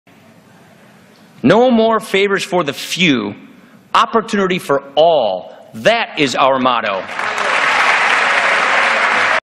Paul Ryan's acceptance speech as Speaker of the House
Paul Ryan addresses the House for the first time as Speaker, "no more favors for the few, opportunity for all. That is our motto"